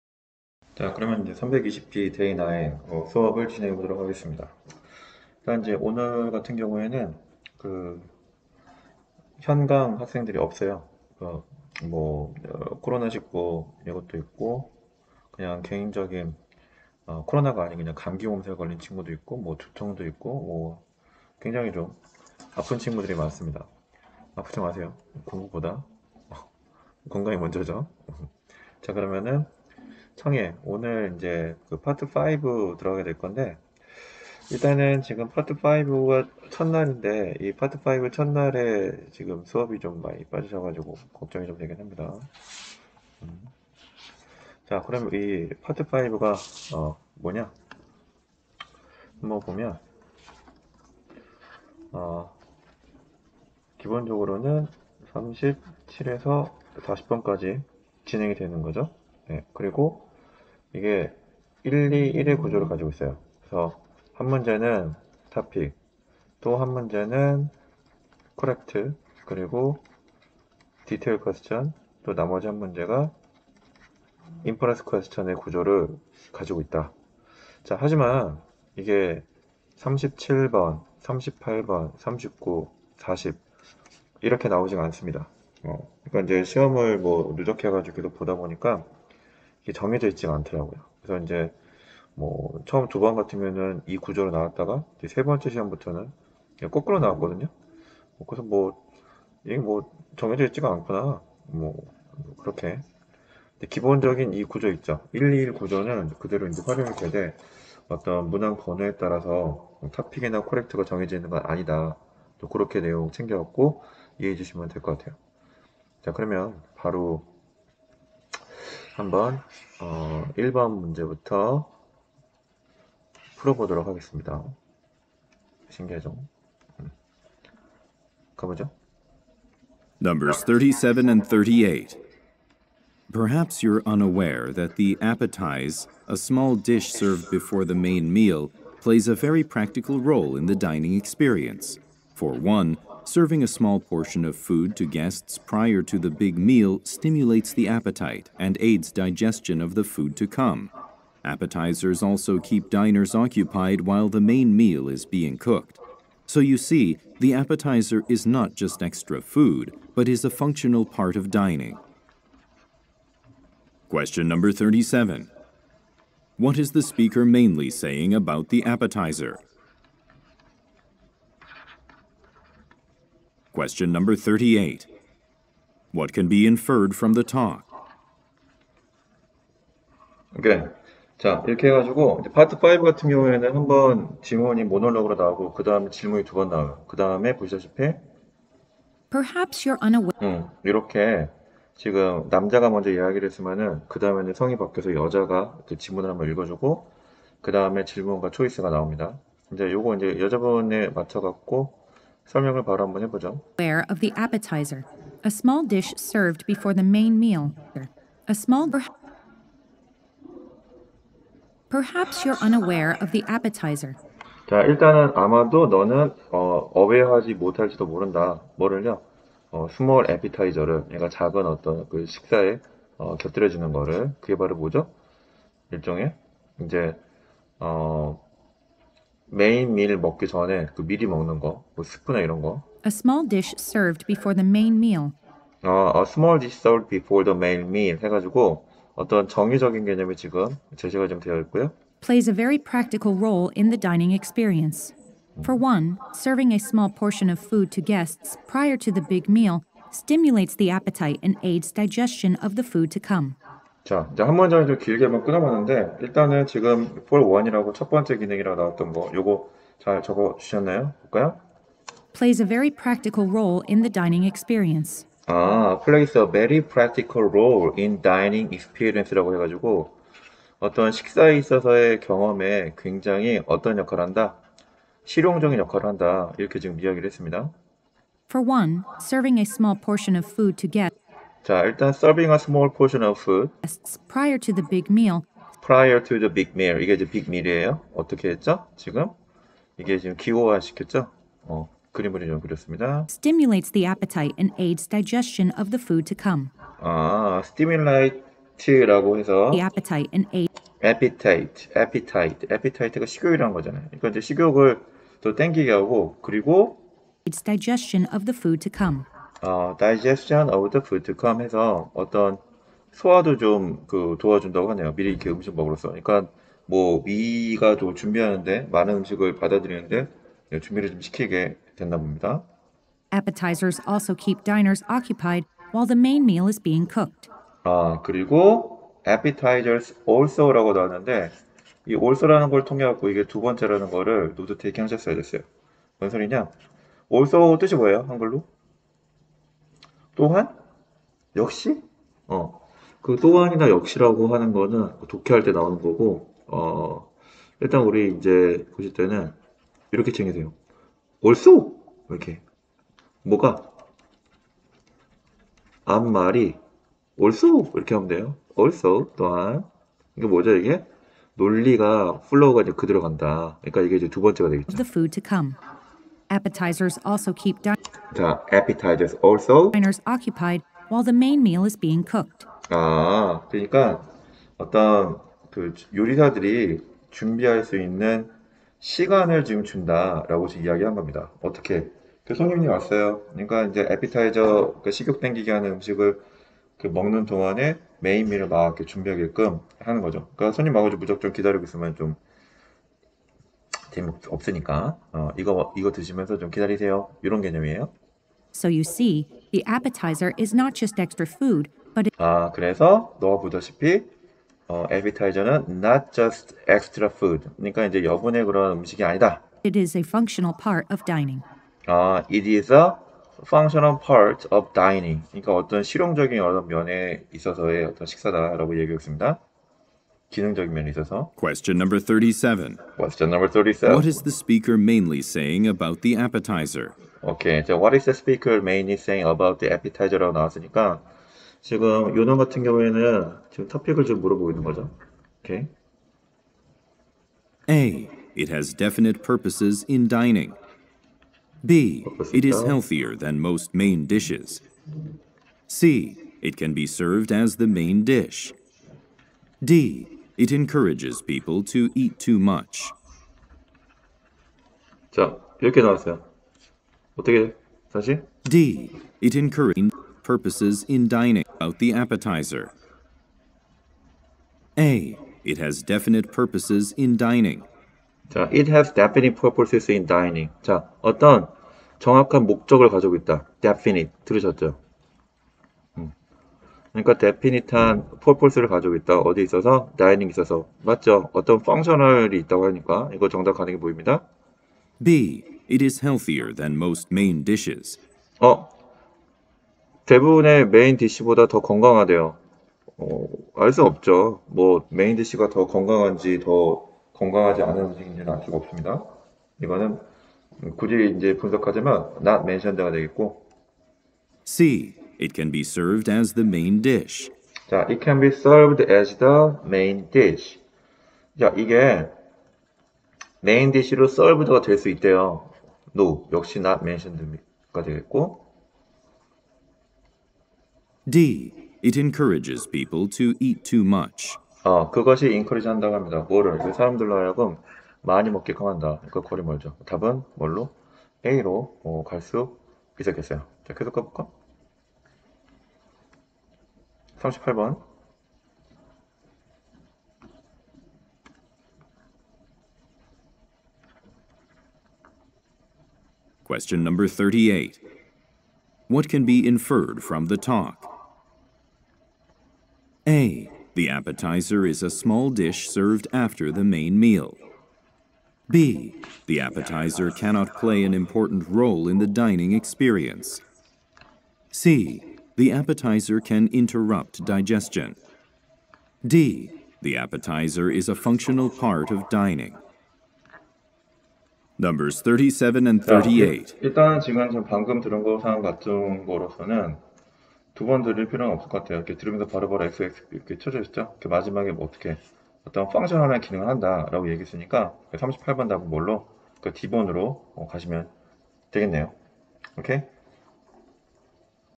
텝스 청해 파트 5 37,38 320B Day 09
다니엘샘의 뉴텝스 청해 해설강의입니다.